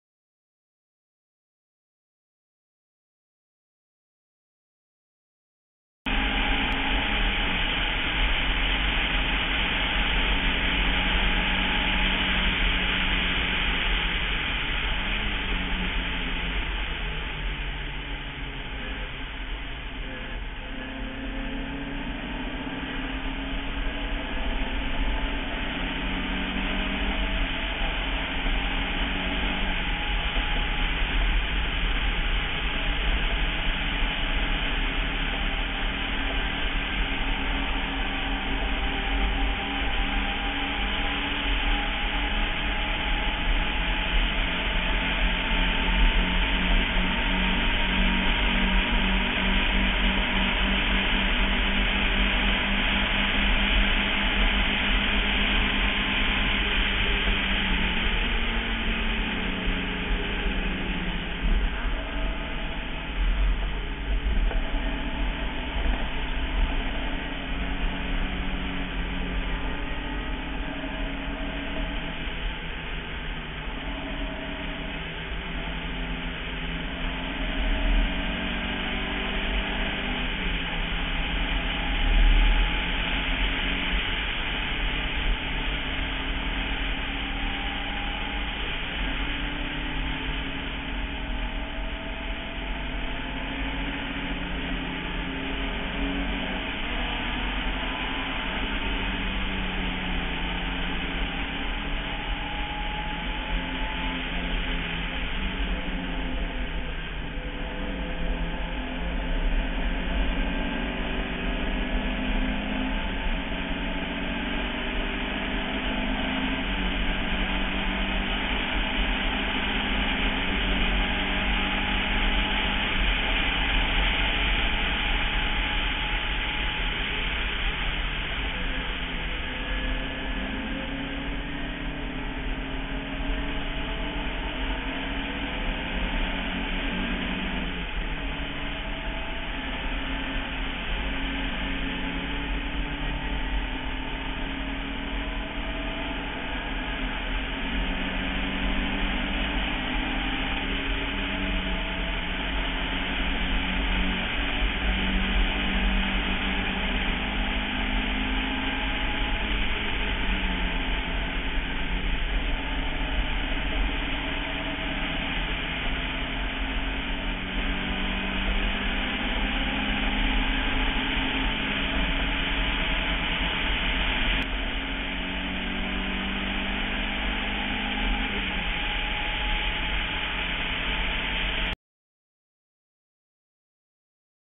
David Conrad at Watkins Glen.m4v
This is David Conrad at a Watkin's Glen race in his MGA